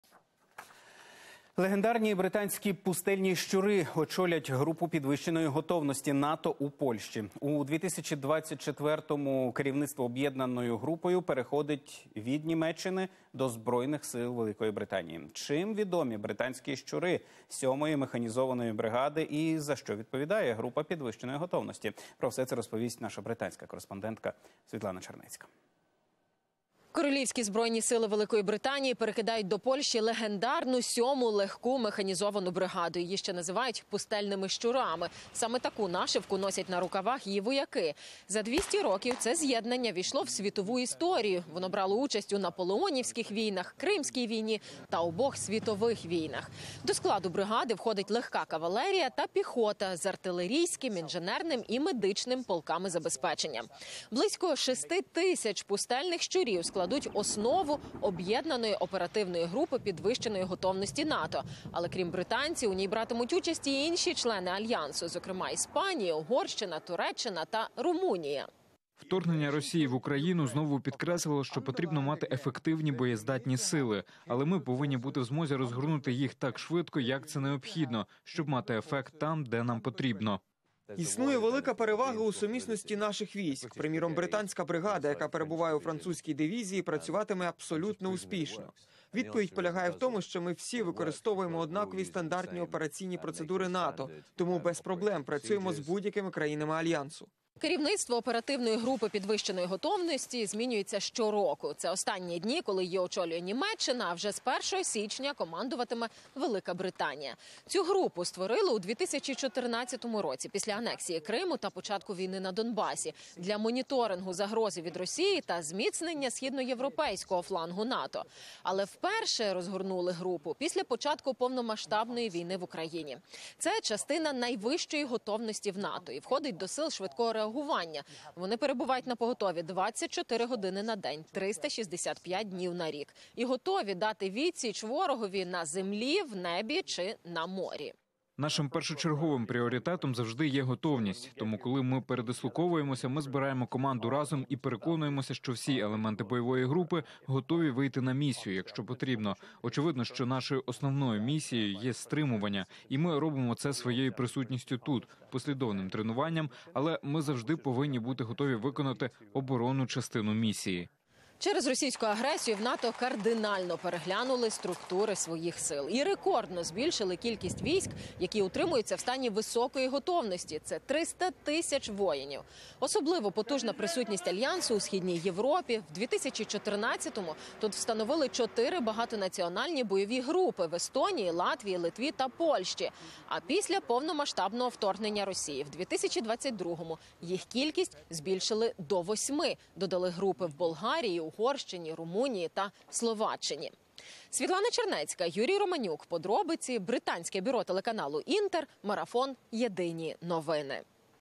Легендарні британські "пустельні щури" очолять групу підвищеної готовності НАТО у Польщі
Чим відомі британські "щури" 7 механізованої бригади та за що відповідає група підвищеної готовності - розповість наша британська кореспондентка Світлана Чернецька.